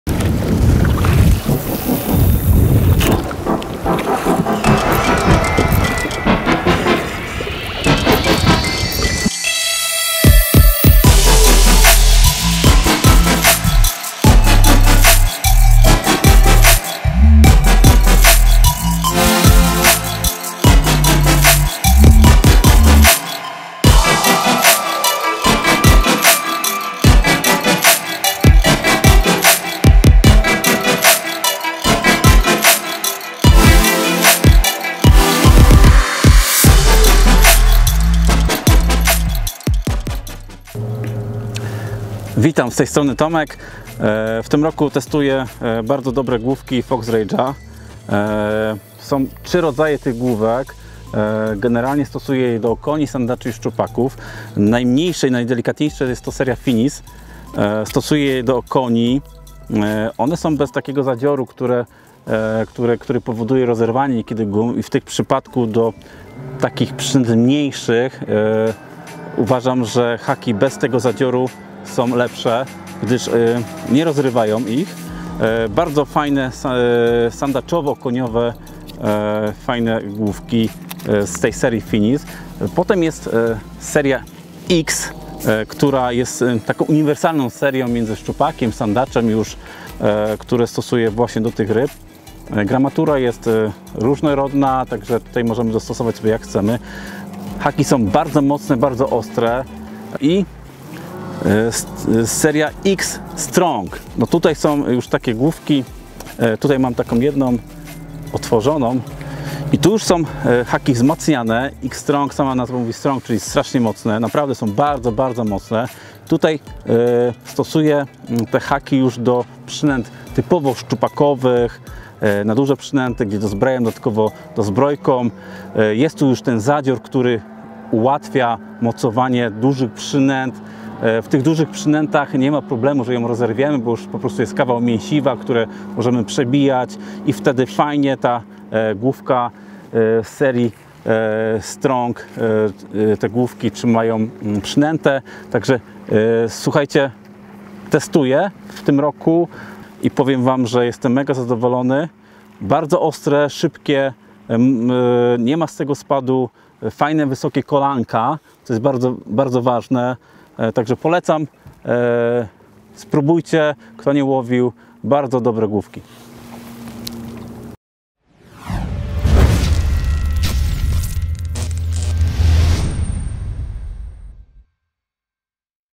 GŁÓWKI JIGOWE Fox RAGE
W ofercie Fox RAGE znajdziecie szeroką gamę główek jigowych, które zostały zaprojektowane z dbałością o każdy szczegół.